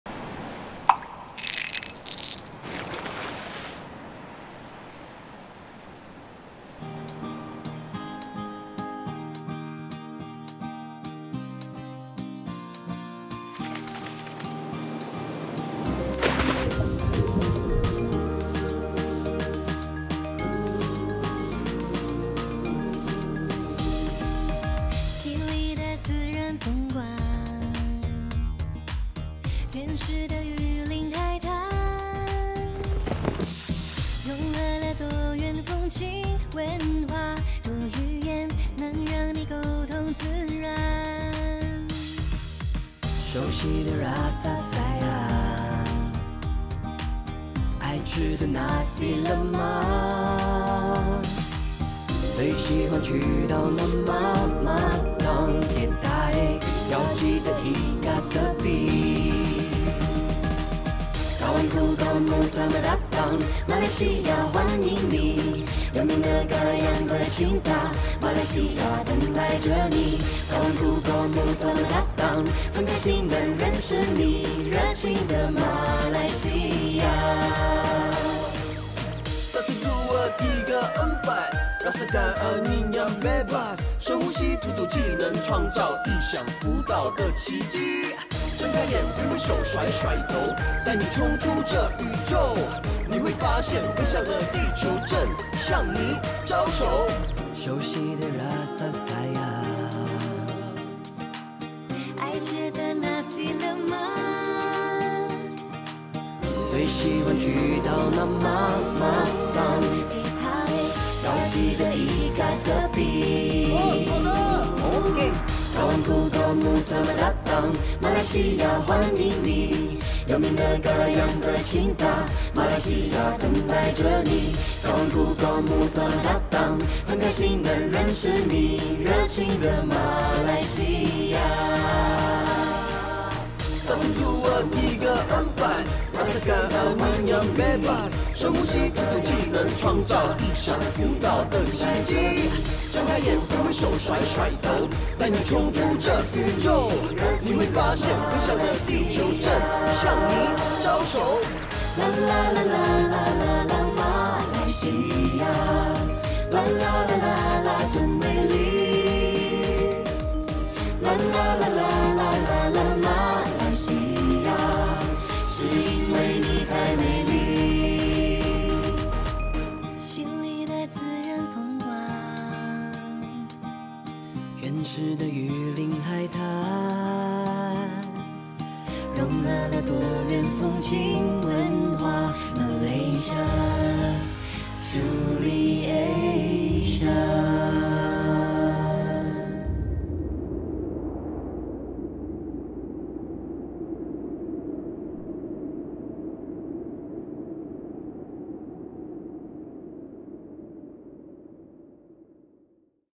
熱情的馬來西亞
2023年即將過去，馬來西亞旅遊促進局(香港分局)為大家呈獻最新馬來西亞旅遊影片，讓您在嚴冬假期下，欣賞馬來西亞風光明媚的景色，暖意盎然，熱情滿載。